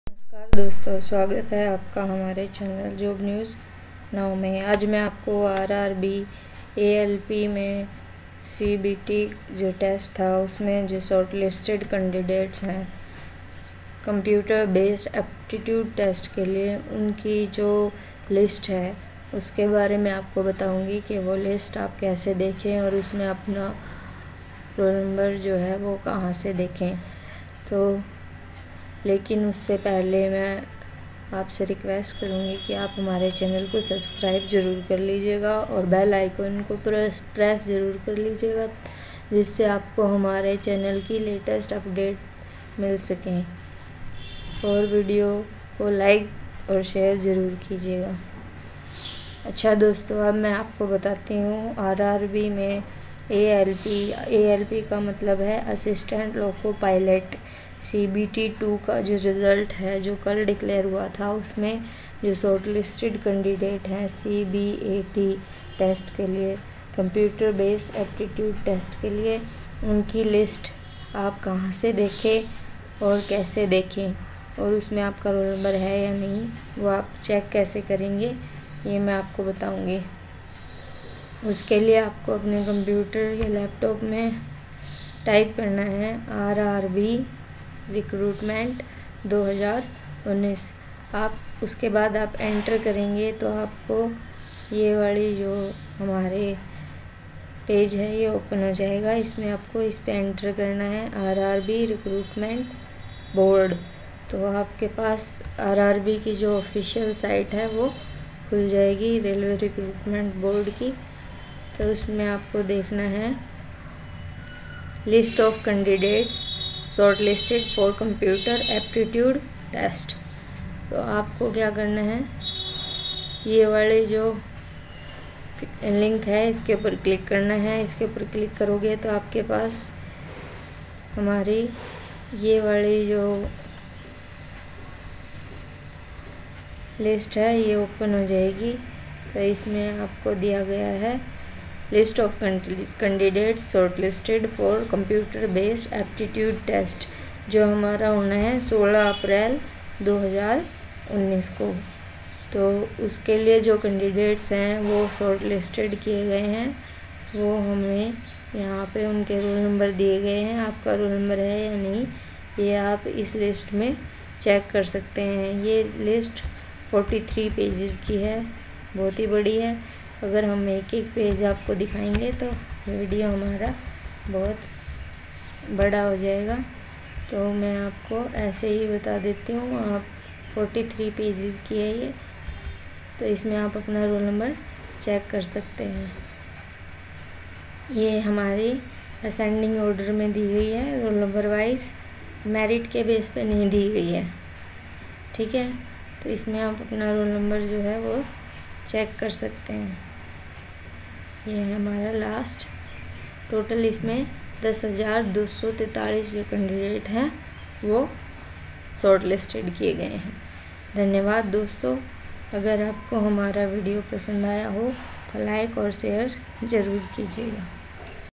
RAILWAY RECRUITMENT BOARD-List of Candidates shortlisted for Computer Based Aptitude Test
GOVERNMENT OF INDIA, MINISTRY OF RAILWAYS, RAILWAY RECRUITMENT BOARD

List of Candidates shortlisted for Computer Based Aptitude Test

Based on the performance in the 2nd Stage Computer Based Test (CBT) conducted on 21-01-19,
22-01-19, 23-01-19 and 08-02-19 by Railway Recruitment Boards for the posts of Assistant Loco Pilot
(ALP) and Technicians against CEN 01/2018, candidates bearing the Roll Numbers listed hereinunder
have been provisionally shortlisted for Appearing in the Aptitude Test scheduled on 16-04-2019.

regards

JOB NEWS NOW